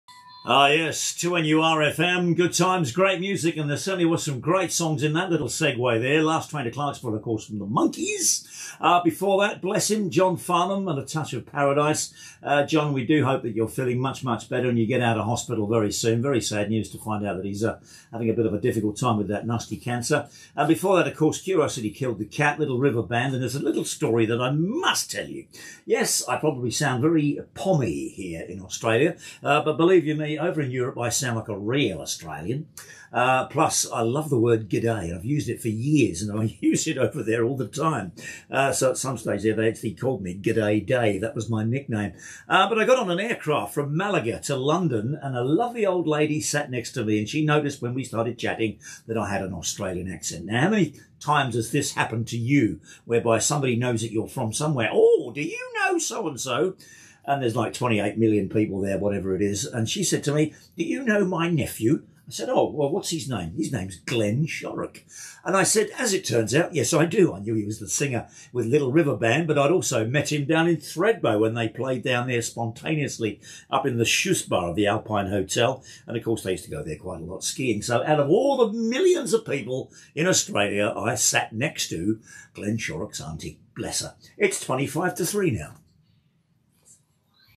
2NURfm Glen Shorrocks Aunty Edited
Just a few radio stories from the studios of 2NURfm for a bit of fun and frivolity this one about Glen Shorrock from the Little River Band!!